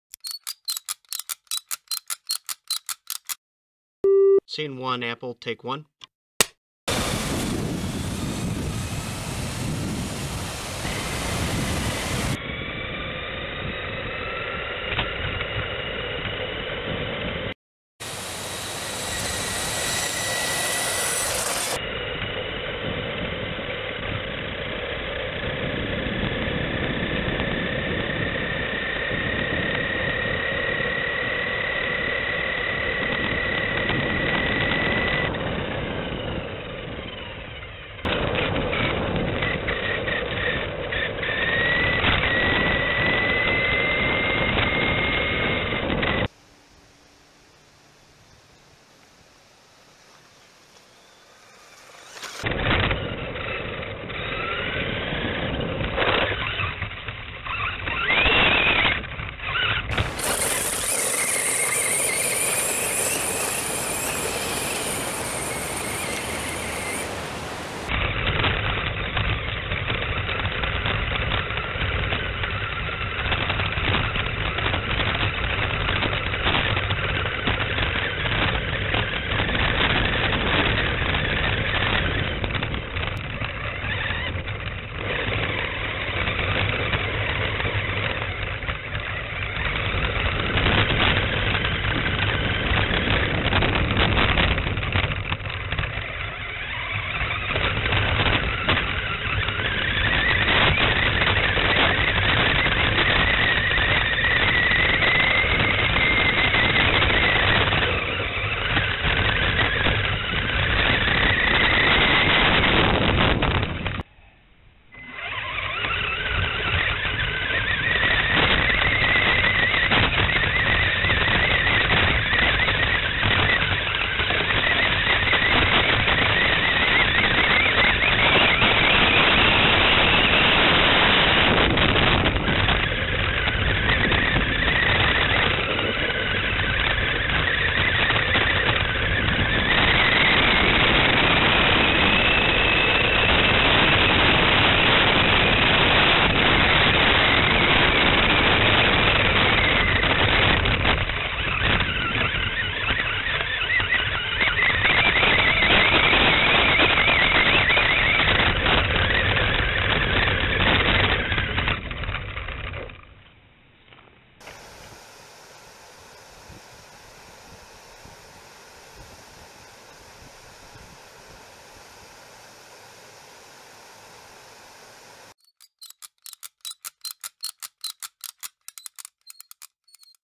WL Toys L969 Autumn FPV Drive
Having FPV fun on a Autumn day in Ohio.